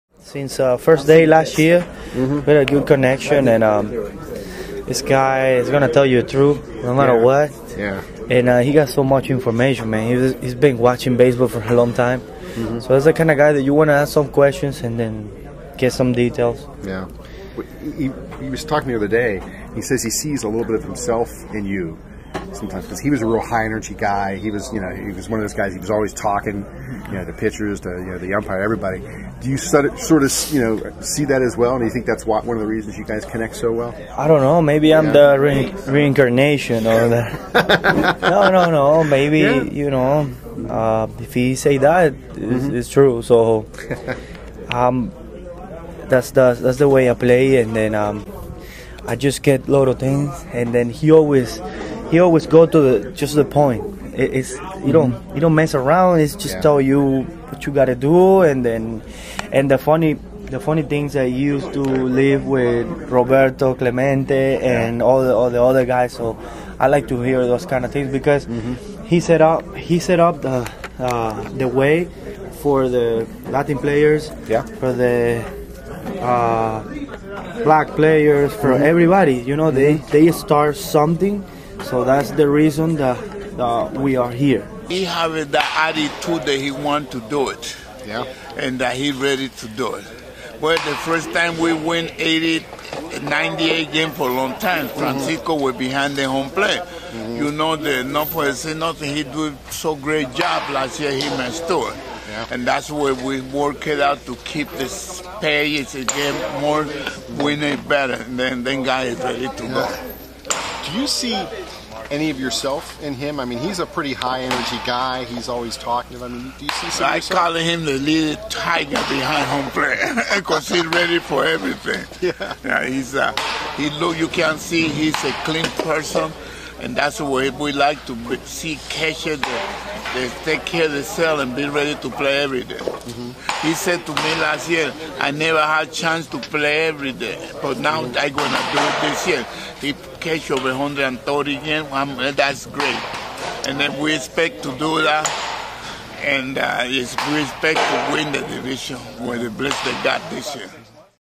Francisco Cervelli and Manny Sanguillen
Pirates catcher Francisco Cervelli and Manny Sanguillen talk to the Trib's Rob Biertempfel at spring training in Bradenton, Fla. Video by: Christopher Horner